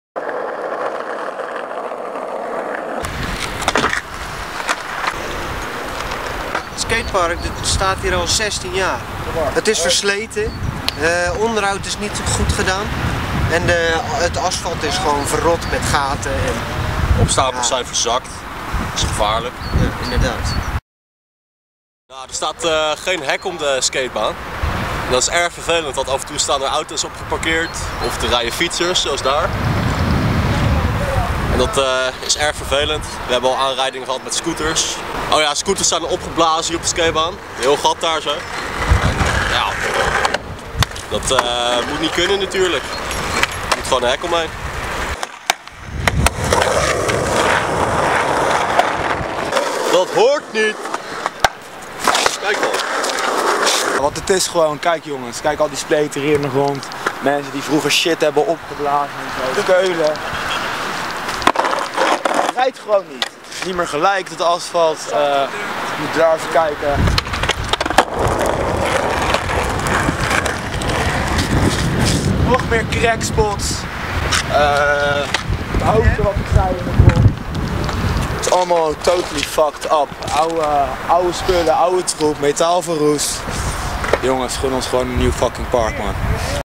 Help skatebaan Sassenheim!!
Help de skatebaan van sassenheim doormiddel van crowdfunding